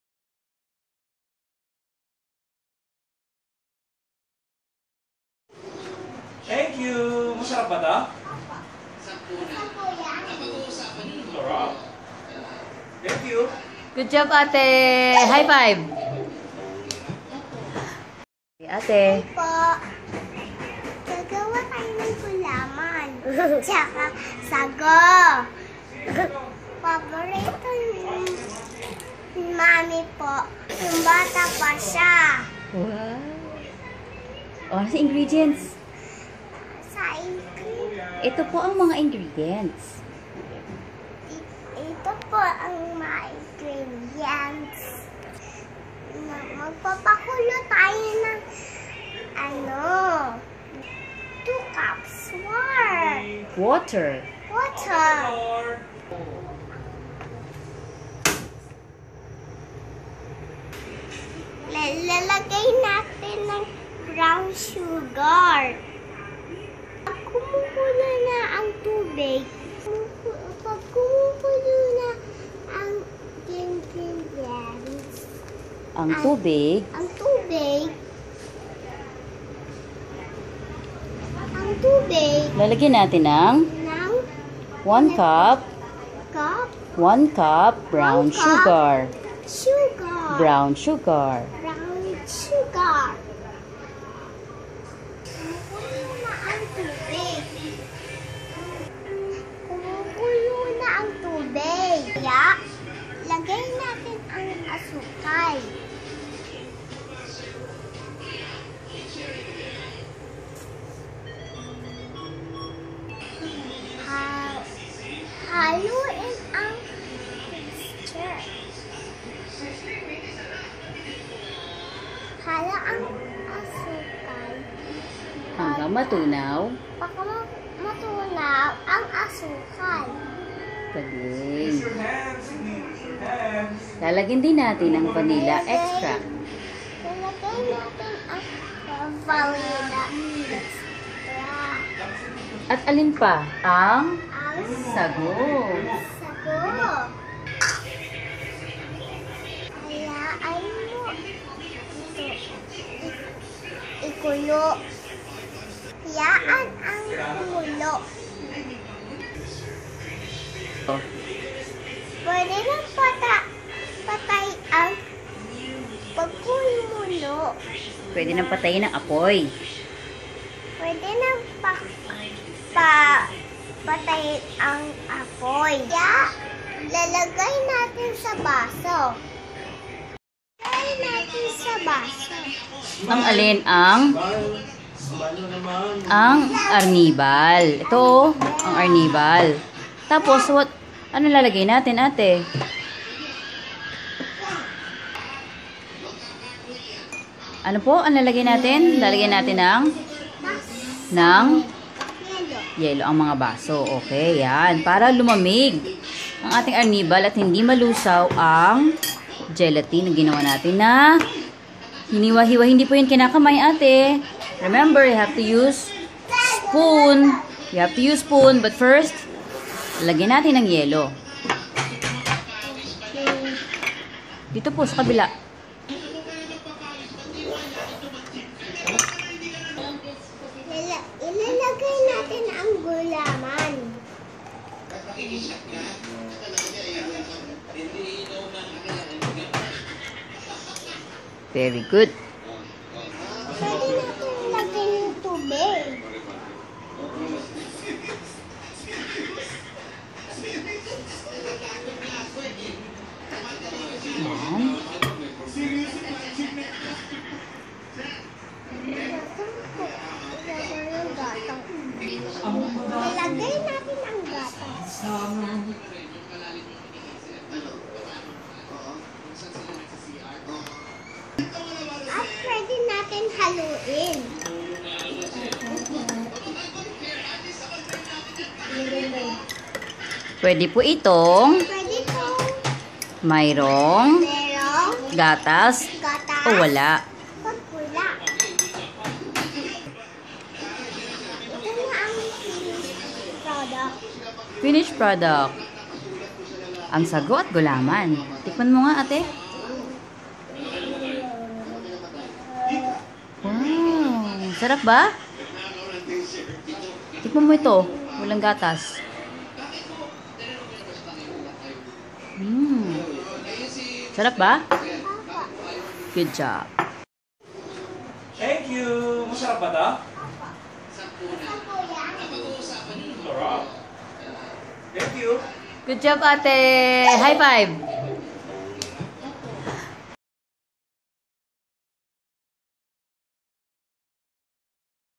My Daughter's SAGO'T GULAMAN recipe
Try my daughter's version of our well-loved sago at gulaman beverage. Thanks so much!